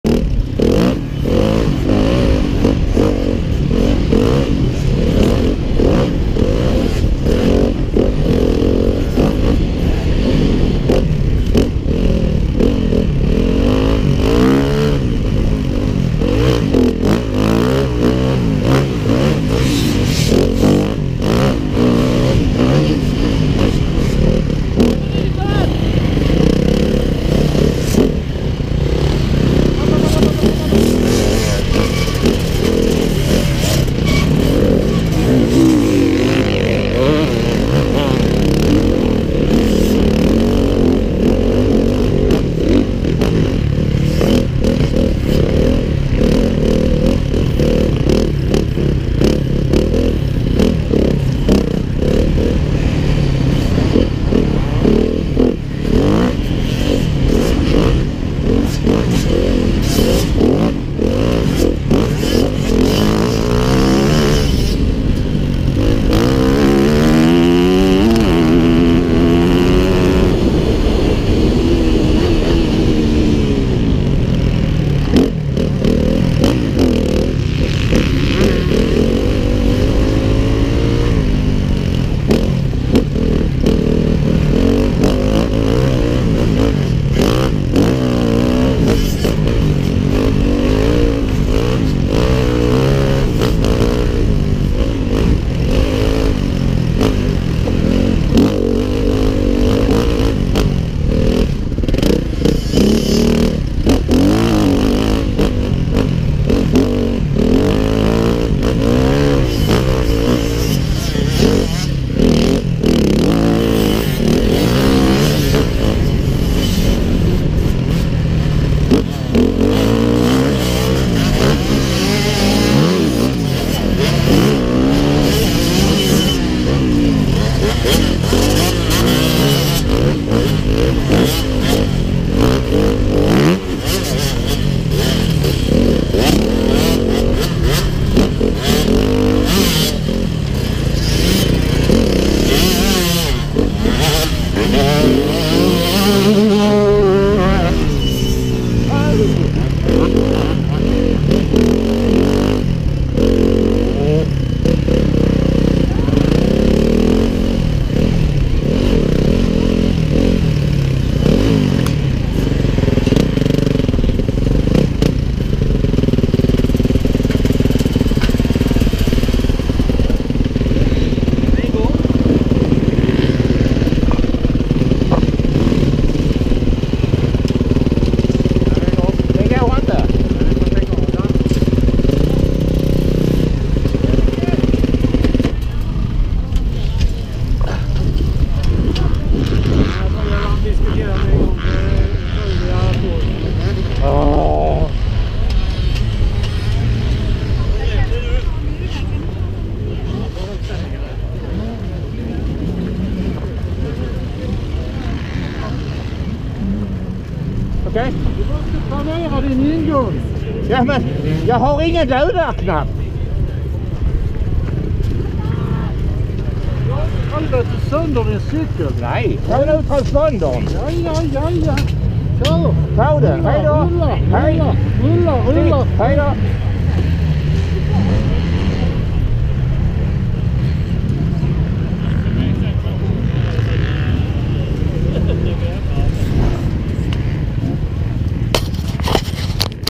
Nyårsstafetten 2016 Del #2
Sista delen av Nyårsstafetten i Ulricehamn in till varvning, i lag med kompis Christer